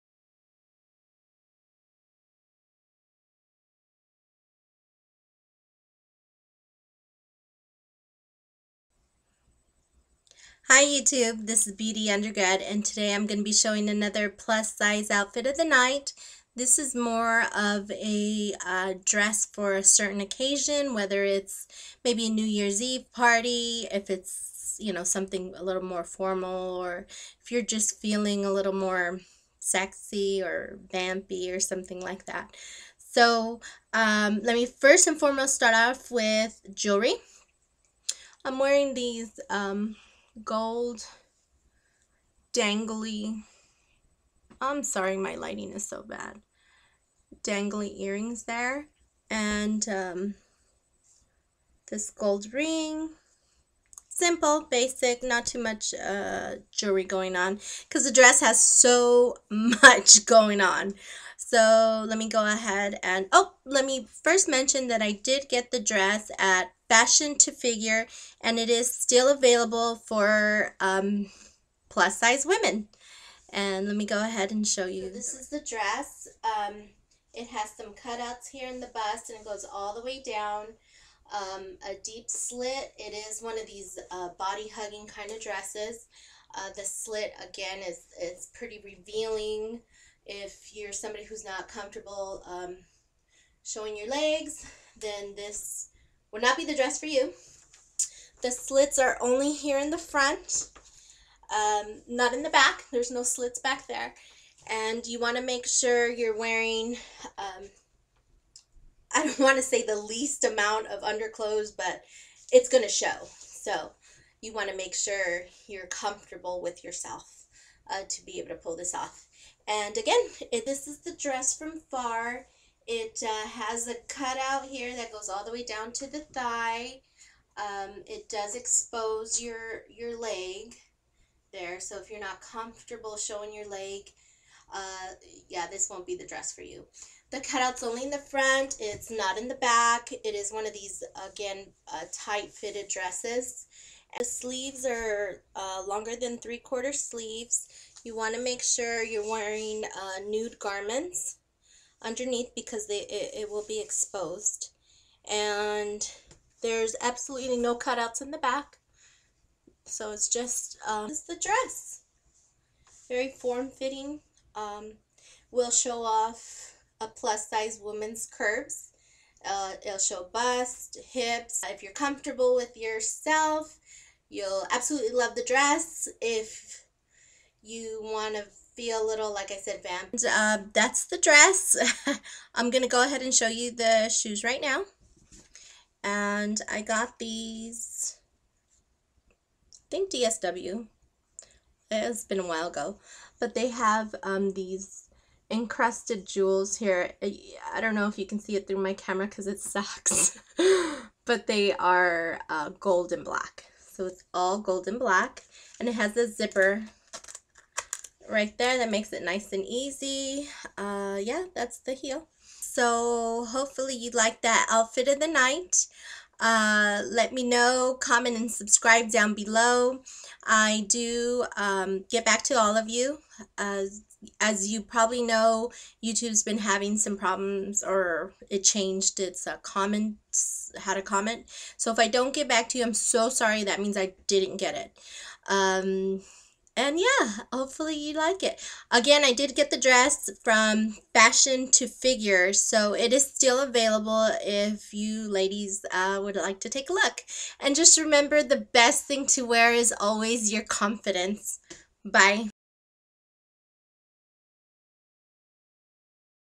Plus Size Sexy Dress
Hi this is beautyundergrad with a plus size OOTN. I am featuring a sexy black dress. A black dress is the most figure flattering dress for any women this dress has cut outs that take this look to a different level.